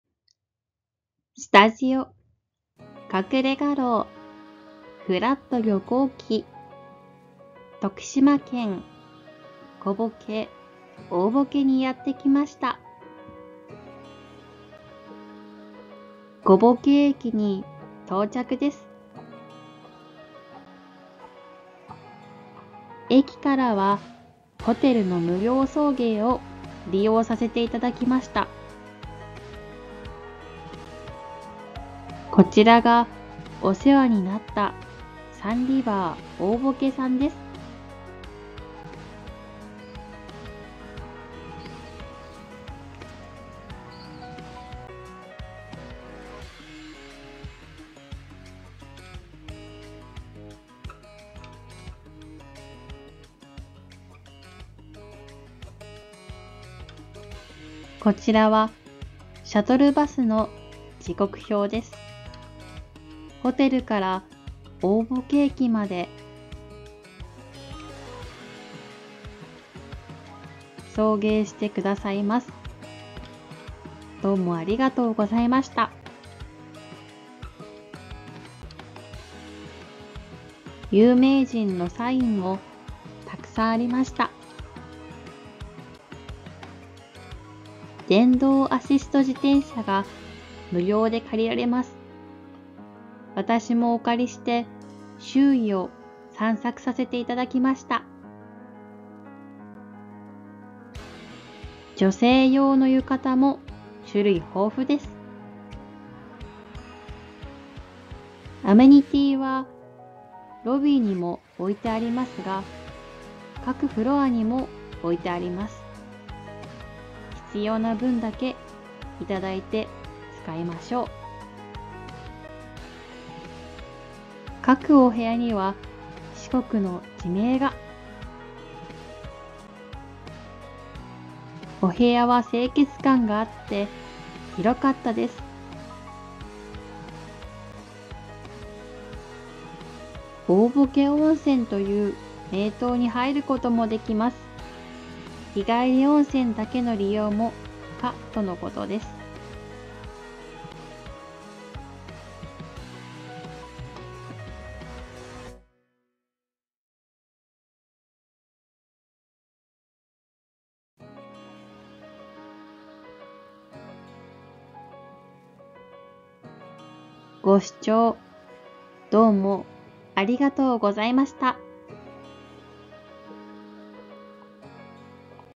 【旅行_008】大歩危＆小歩危_02 / Oboke＆Koboke_02：旅行記 / Travel
徳島県の大歩危(おおぼけ)&小歩危(こぼけ)です。
今回の動画は、お世話になった「サンリバー大歩危」さんのご紹介です。
無料シャトルバス、電動アシスト自転車や浴衣、大歩危温泉、散策マップ…大変助かりました。
親切にして下さったスタッフさん、どうも有難う御座いました。
感謝の気持ちを込めて、動画で紹介させて頂きました。

【スタジオ_隠れ画廊】 @Studio_KakureGarou
撮影：ふぁーすとおふぃさー / First Officer
ナレーション：芹澤 ロカ / Roca SERIZAWA